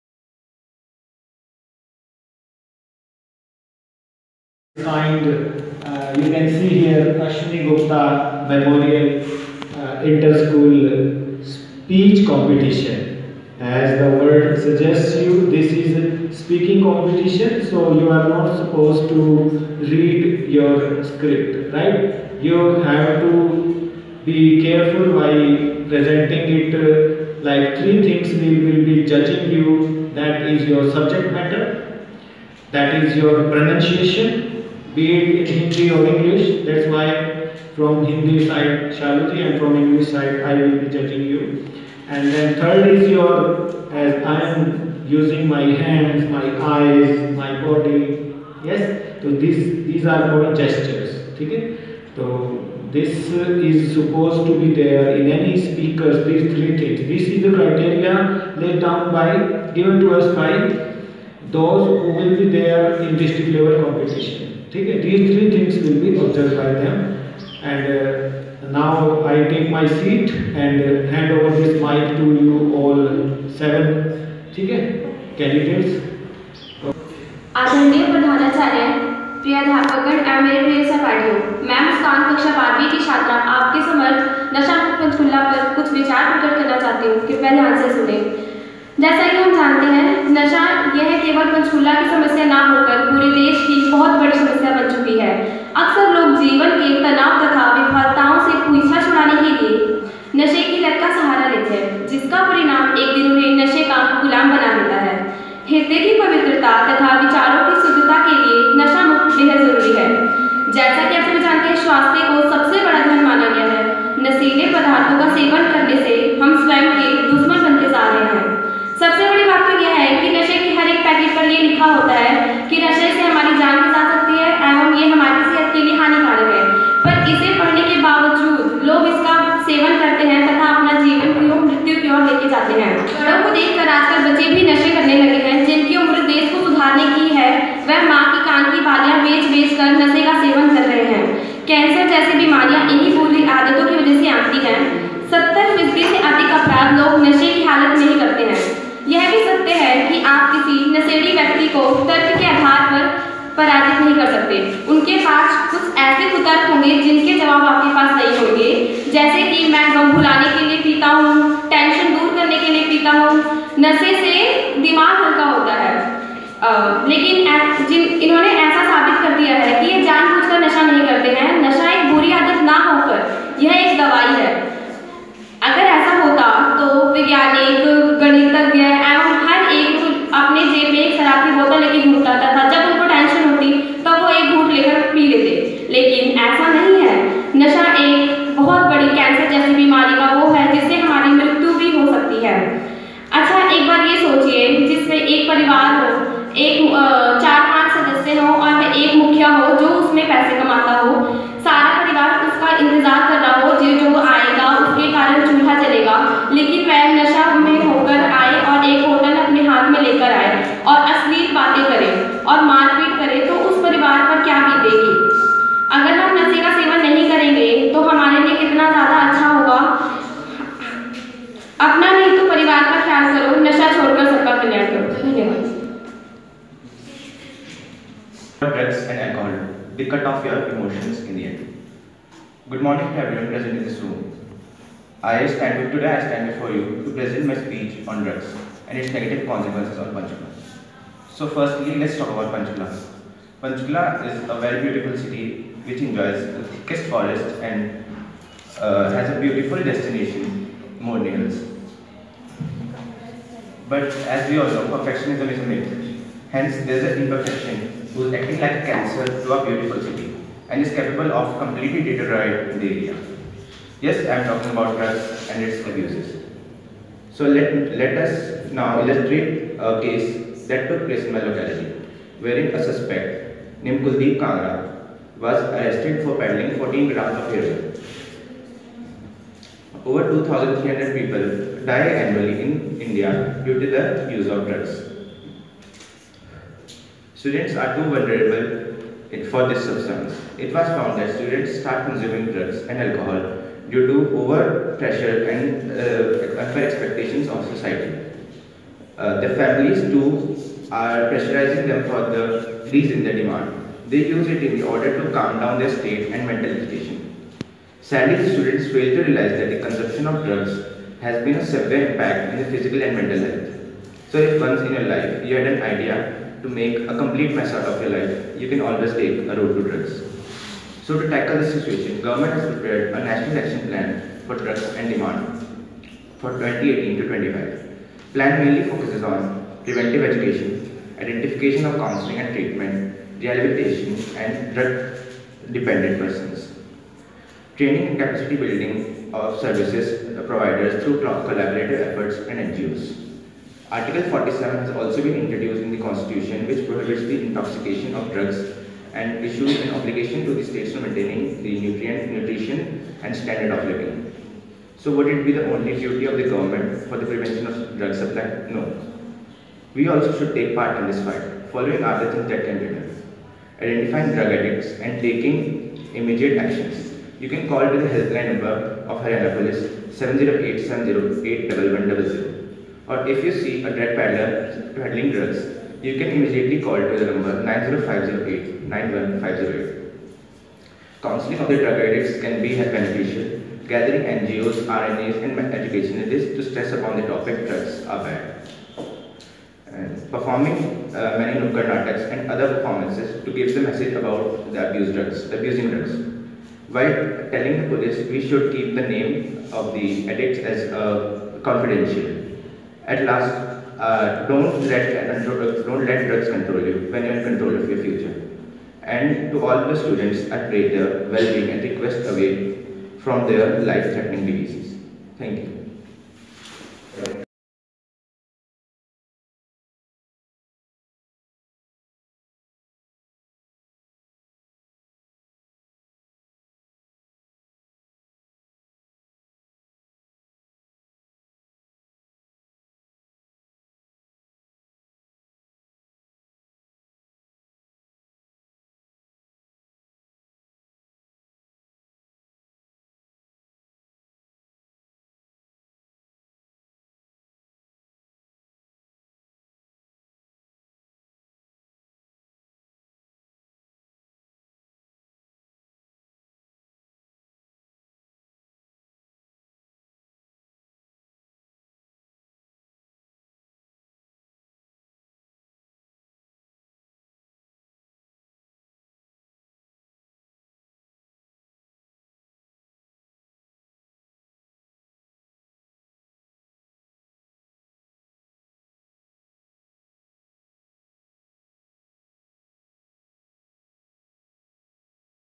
speech on drug free panchkula by vijay kumar
In this video, two speeches are given on how to make PANCHKULA, a drug free city by vijay kumar